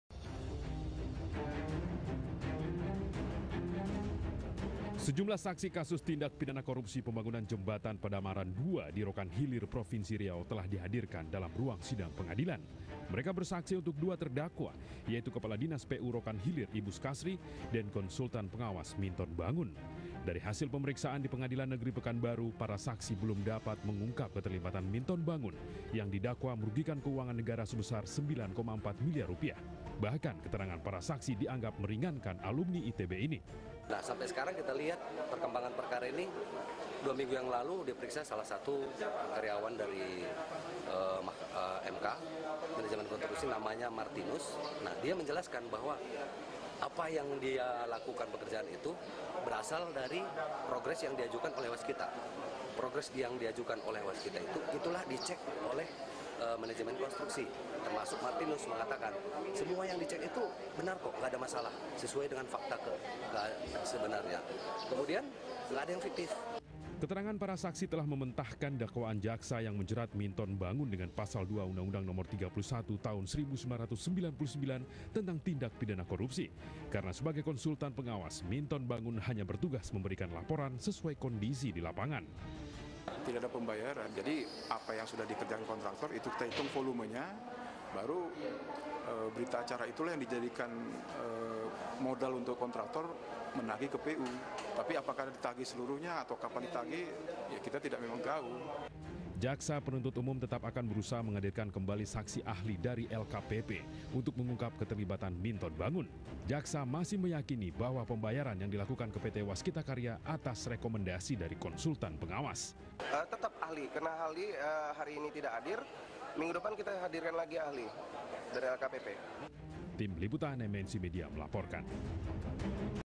Sidang Kasus Korupsi Jembatan, Saksi Ringankan Minton Bangun
Sidang kasus korupsi Jembatan Pedamaran II di Rokan Hilir Provinsi Riau, kembali digelar di Pengadilan Negeri Pekanbaru. Jaksa kembali menghadirkan saksi-saksi untuk membongkar keterlibatan terdakwa mantan Kepala Dinas Pekerjaan Umum Kabupaten Rokan Hilir, Ibus Kasri dan konsultas pengawas proyek, Minton Bangun. Para saksi belum bisa mengungkap keterlibatan Minton Bangun dalam kasus tersebut. Bahkan, keterangan mereka dianggap meringankan Alumni ITB itu.